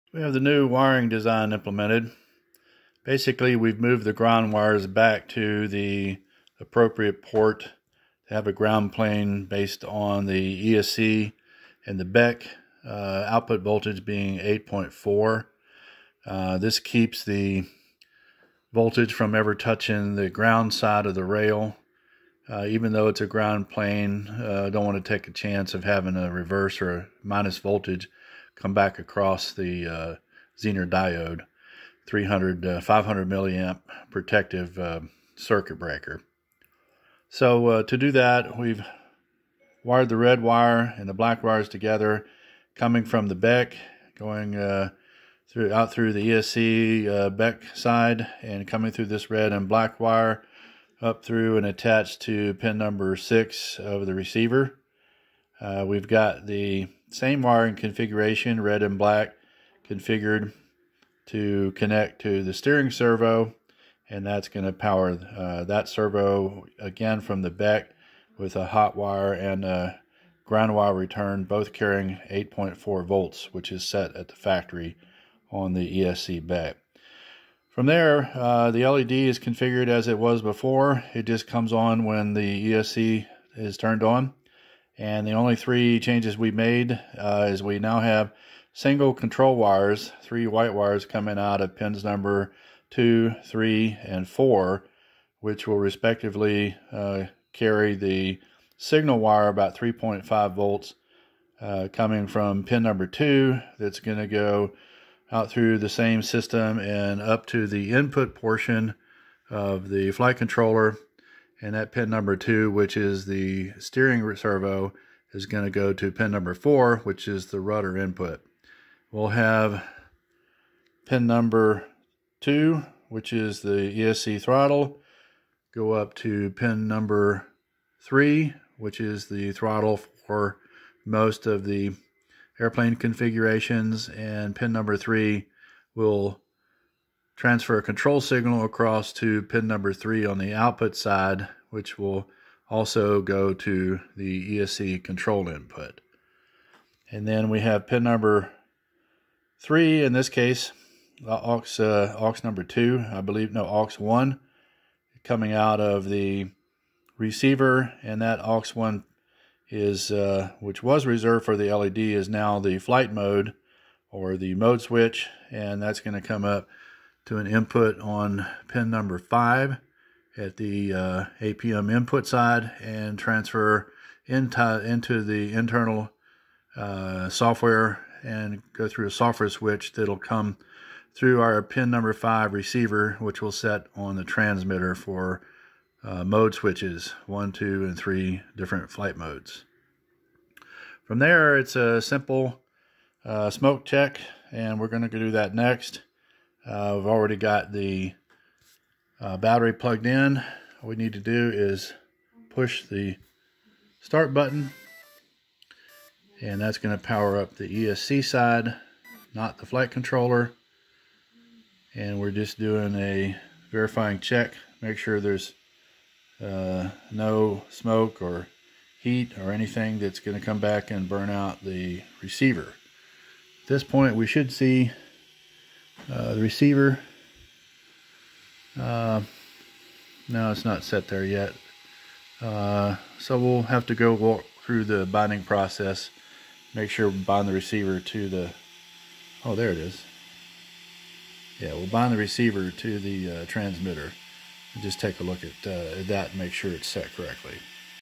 Reroute ESC power complete connected to chassis frame space on LOSI Buggy
Series of Documentary Videos describing the design, build and test of the autonomous LOSI Desert Buggy.